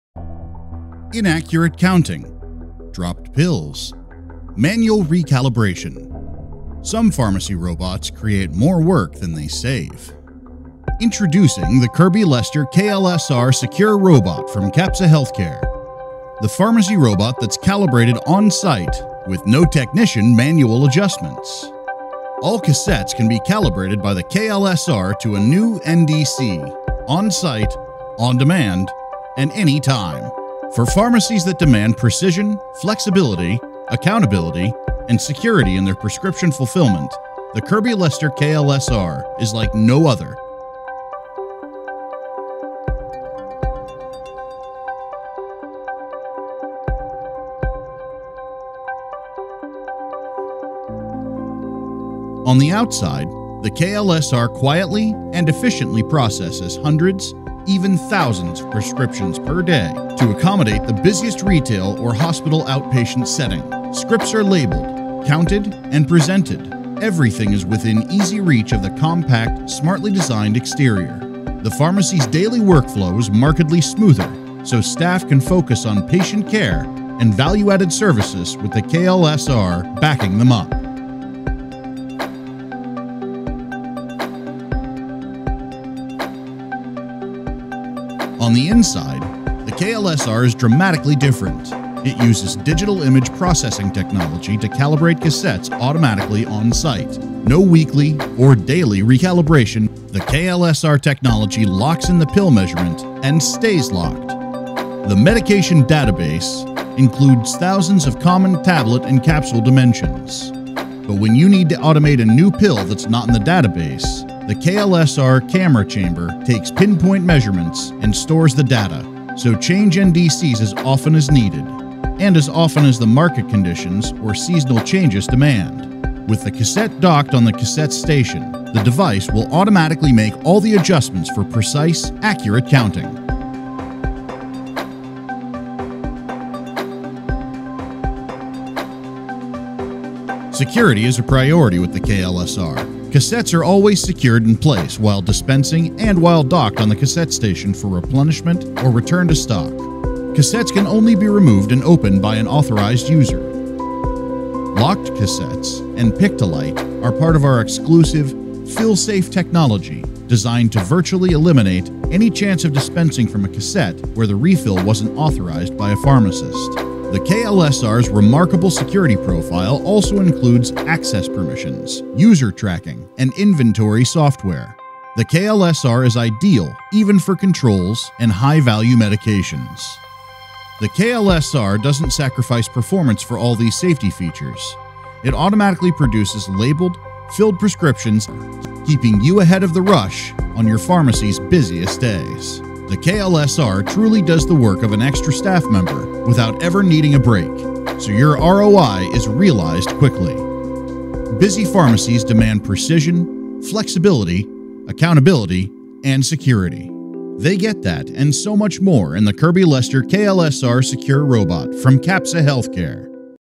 KL SR Product Overview
The Kirby Lester KL-SR, from Capsa Healthcare, is a pharmacy robot unlike any other, with important innovations inside and out. Cleverly designed cassettes and digital image processing technology requires absolutely no technician calibration or maintenance for any cassette – whether it’s a drug that is already in the database or a new NDC. Never need to order a new cassette, and never spend a minute recalibrating. There’s virtually no chance for human error when adding a new medication, and no miscounts.
The KL-SR easily fits into your existing floor plan and fully manages 108 medications. With the Kirby Lester KL-SR effortlessly automating the fast-movers, the pharmacy staff is freed up to focus on value-driving initiatives such as adherence, MTM, and patient counseling.